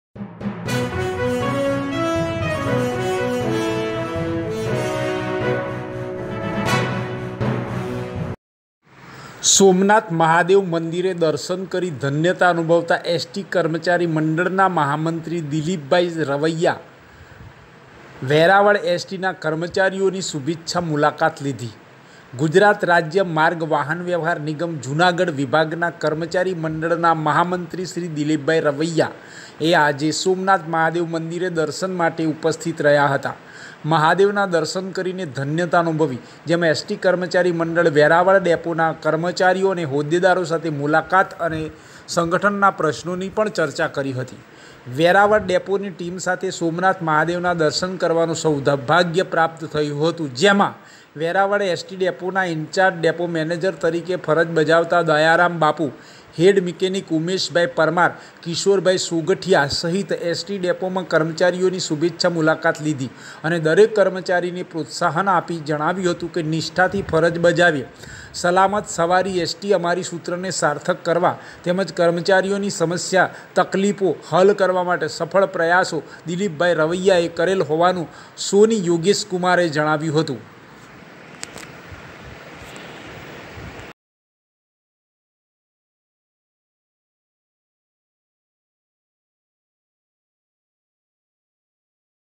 રાજ્ય માર્ગ વાહન વ્યવહાર નિગમ જુનાગઢ વિભાગના કર્મચારી મંડળના મહામંત્રી સોમનાથ મહાદેવ મંદિર દર્શને
𝗟𝗮𝘁𝗲𝘀𝘁 𝗡𝗲𝘄𝘀